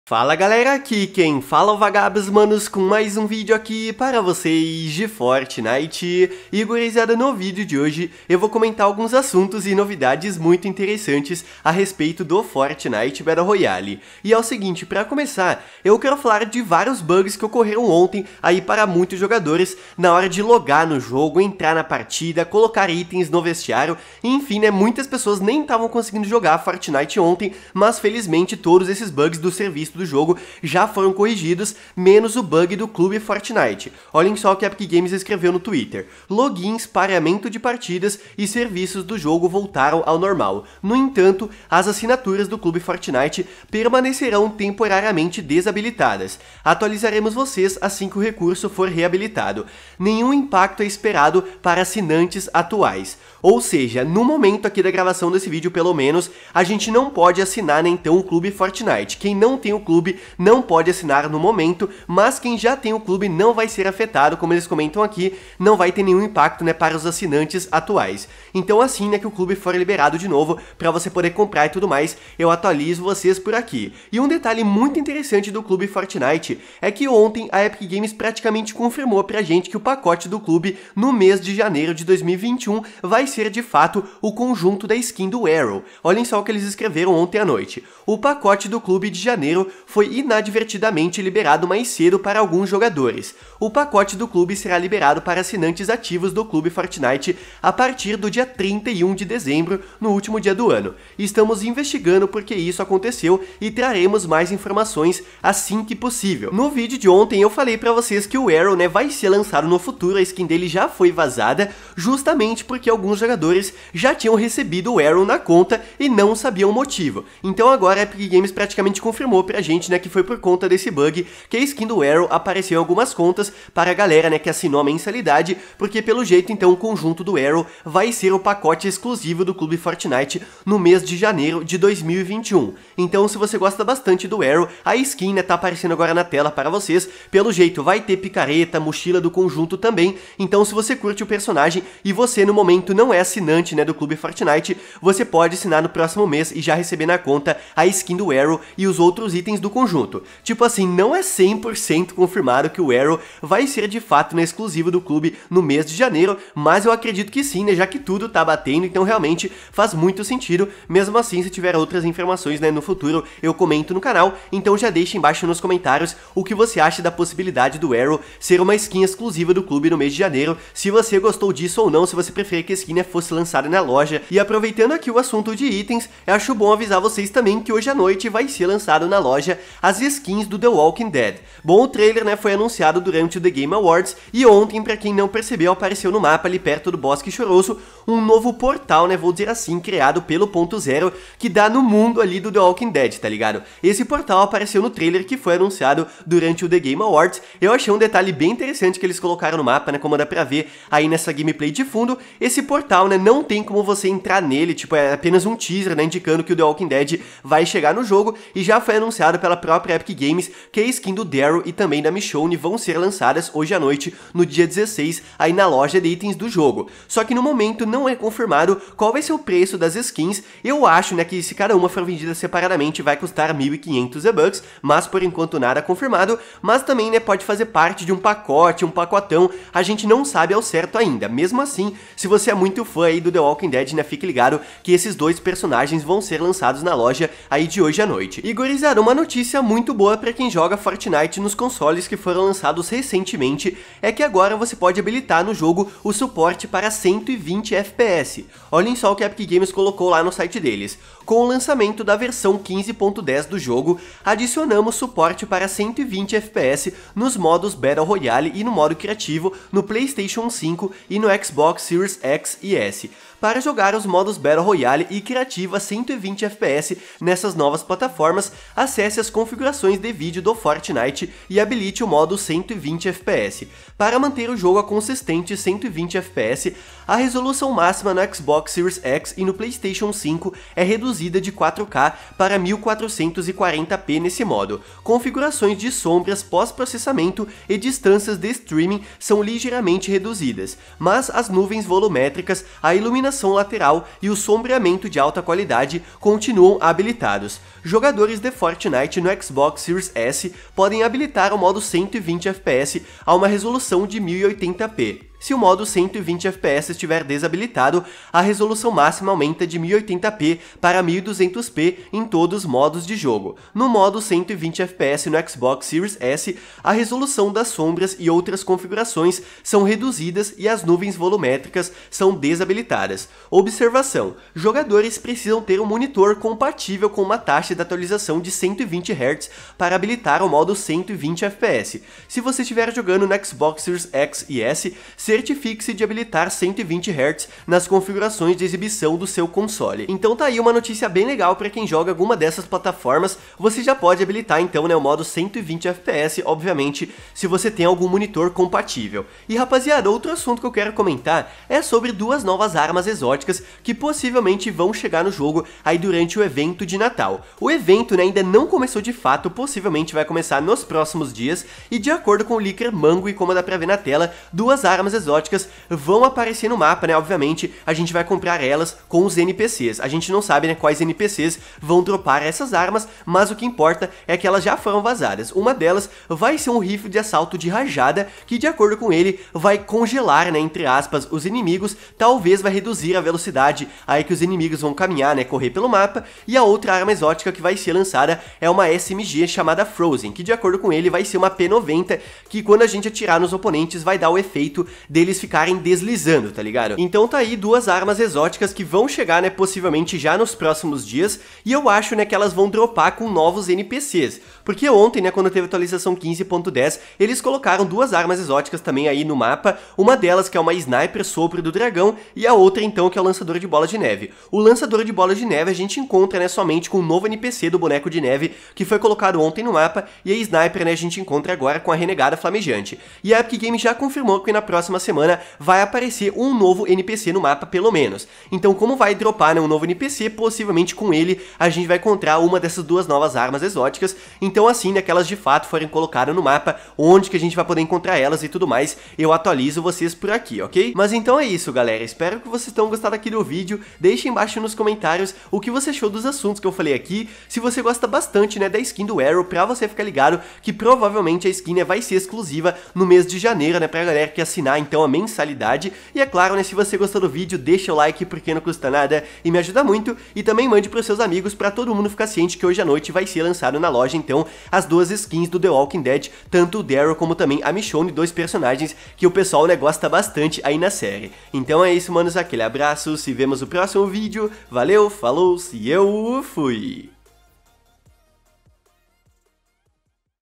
FORTNITE - CLUBE DESABILITADO, LOJA THE WALKING DEAD e SKIN ARROW?
Fala gurizada, nesse vídeo comento novidades e noticias do Fortnite Battle Royale, sobre clube Fortnite desabilitado, skin Arrow, skins The Walking Dead hoje na loja e mais. Espero que gostem :)